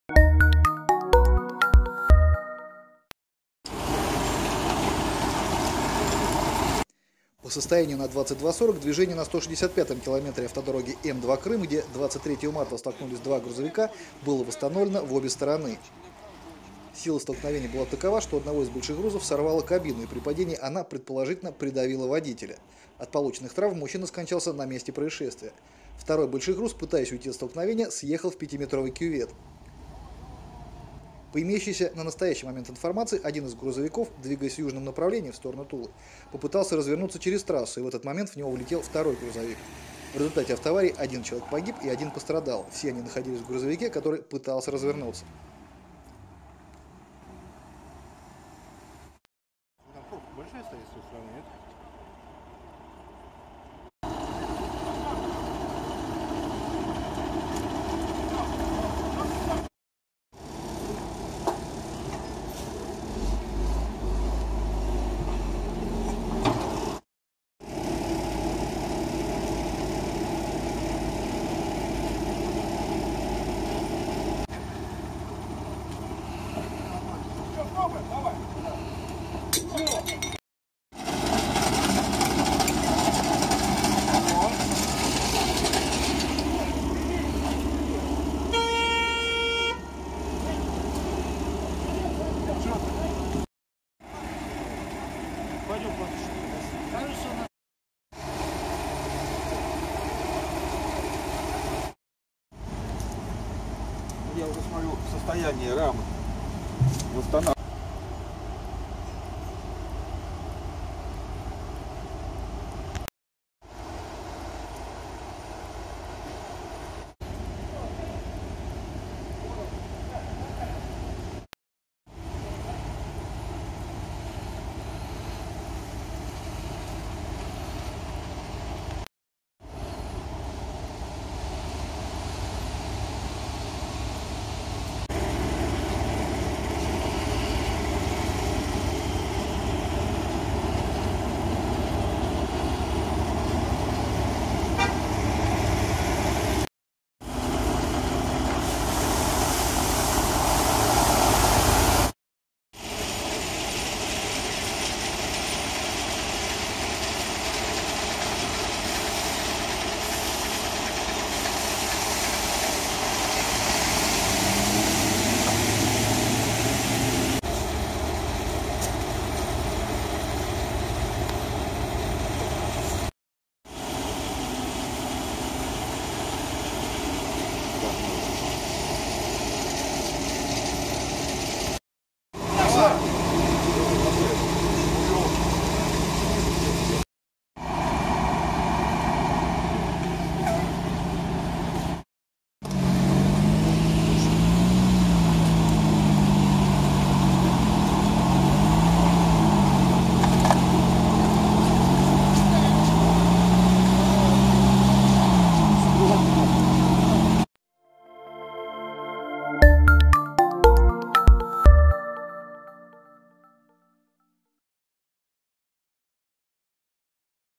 Страшное ДТП С двумя фурами на М-2 под Тулой
По состоянию на 22:40 движение на 165 км автодороги М-2 Крым, где 23 марта столкнулись два грузовика было восстановлено в обе стороны.

Сила столкновения была такова, что у одного из большегрузов сорвало кабину и при падении она, предположительно, придавила водителя. От полученных травм мужчина скончался на месте происшествия.

Второй большегруз, пытаясь уйти от столкновения съехал в пятиметровый кювет .

.По имеющейся в настоящий момент информации, один из грузовиков, двигаясь в южном направлении (в сторону Тулы ) попытался развернуться через трассу и в этот момент в него влетел второй грузовик.

В результате аварии, один человек погиб и один пострадал. Все они находились в грузовике, который попытался развернуться.